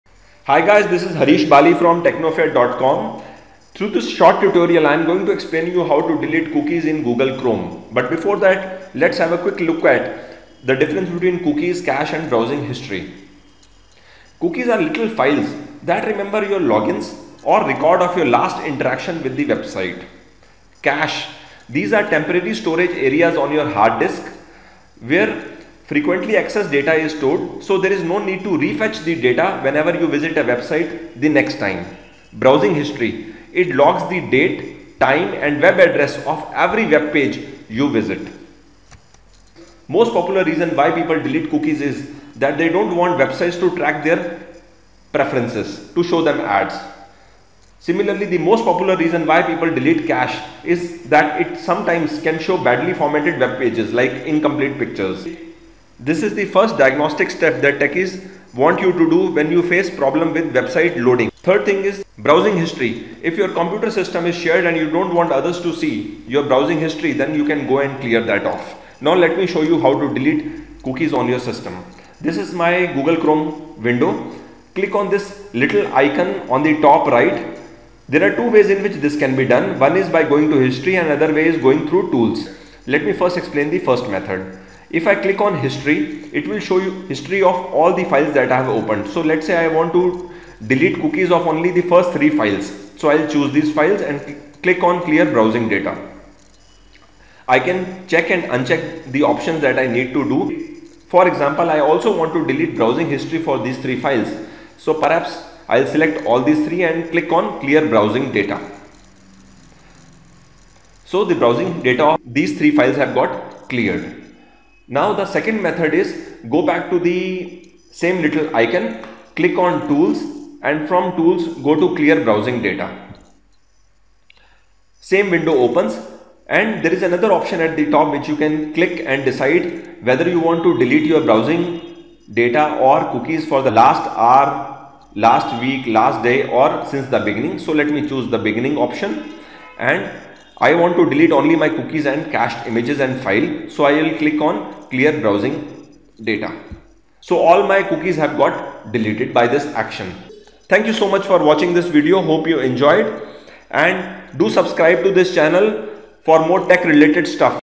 How to Clear Cache and Delete Cookies on Google Chrome?
This tutorial explains how to clear cache, delete cookies and browser history on Google Chrome.  People generally mix between these 3 concepts, while they are completely different. I thought of sharing the concept along with sharing the method to delete them in Google chrome.

What are cookies – These are small text files, generally used by e commerce companies to track user preferences, so that they can show ads for product for which users have shown interest. Also  it is a tool through which websites recognize their customers.
If you fill up some information regarding you sign up in e commerce site and suddenly that link breaks, when you come back to it, you will notice that information already filled is there, this happens largely because of cookies.

Most popular reason people delete cookies is because they don’t want websites to track their preferences.

What is cache – These are temporary files on your hard disk, that get get created, so that website loading gets faster and next time when you visit a website, it loads from your computer system, unless some changes have happened during this period.

 Popular reason why people delete or clear cache is because – they are facing problems in loading the latest version of website or website with updated information. They may do so if they are facing problem in loading images.

What is browser history – Every web page you visit is tracked by Google chrome and a history of various details of web browsing is maintained on server through your system. If you don’t want Google to track your browsing history then you can delete the same.

Generally it is observed that if someone does not want his browsing history to be viewed by others, especially when using shared computer then people go to history and delete the web pages visited by them or they accidentally end up deleting the complete browsing history.  This happens may be because they do not know that browsing history of individual web pages can also be deleted.

This tutorial explains process to clear cache, delete cookies and browser history in Google chrome.

Suggested watch – if you wish to delete your facebook account, I have a short tutorial for the same

If you found this tutorial good and interesting, then do share it with your friends so they also learn how to delete cookies, browser history and cache on Google chrome browser.